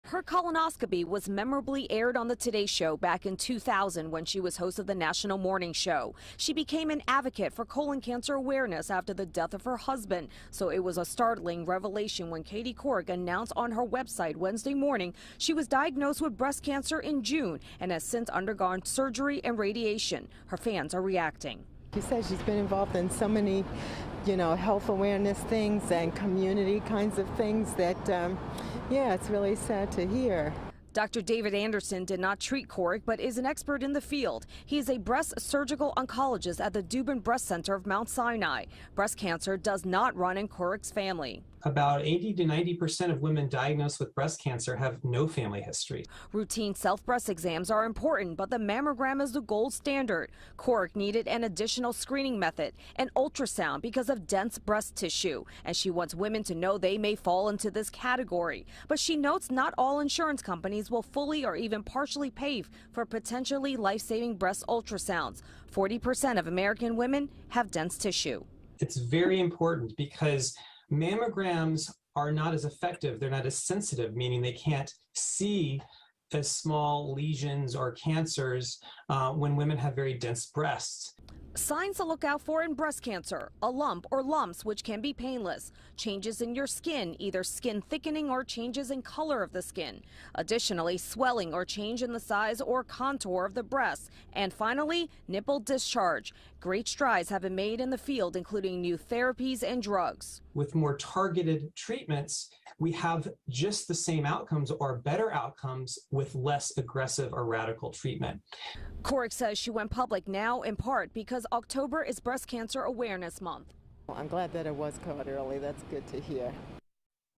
Katie Couric discloses breast cancer diagnosis
Journalist Katie Couric announced that she was diagnosed with breast cancer in June and completed radiation earlier this month.